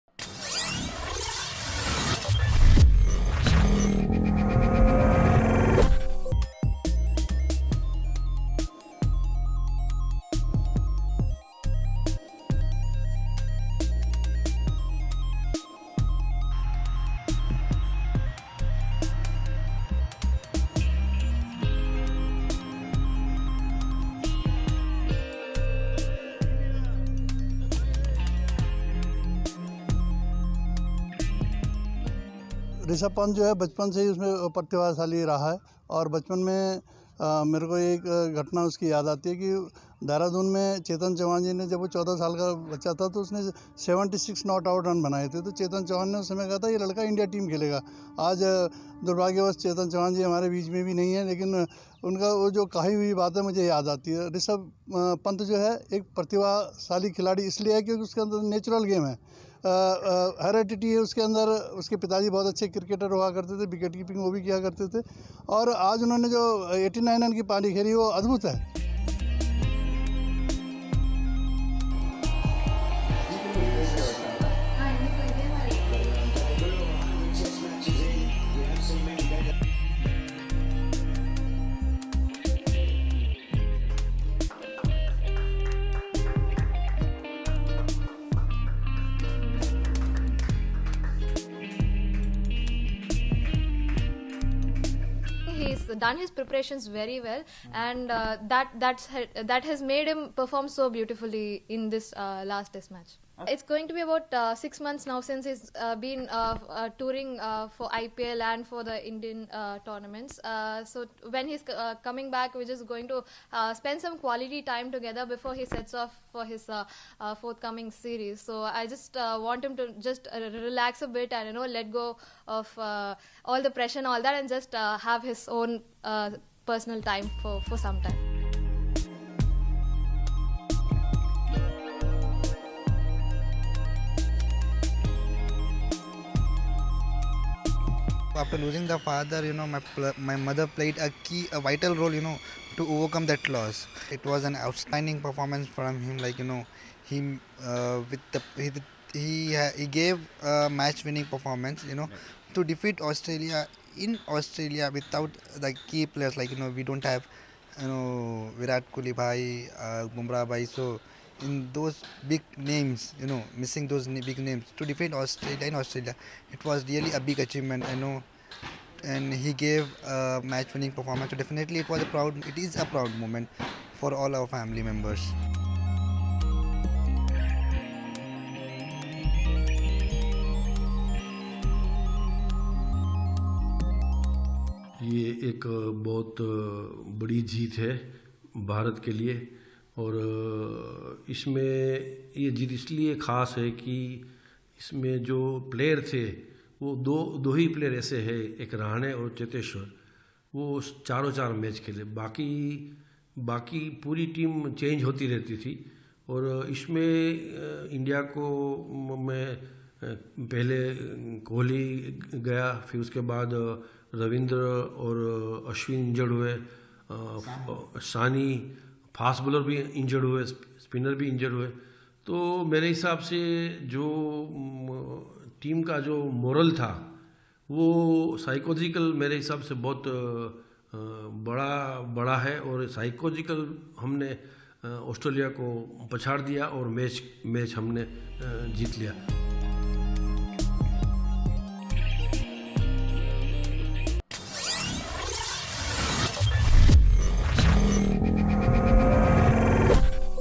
ऑस्ट्रेलिया की जीत से सुंदर, ऋषभ और सेराज का परिवार खुश
ऑस्ट्रेलिया की जीत से सुंदर, ऋषभ और सेराज का परिवार खुश, क्योंकि जितना शानदार प्रदर्शन इन लोगों ने किया है…उसकों को देखकर सभी लोग खुश हो सकते है…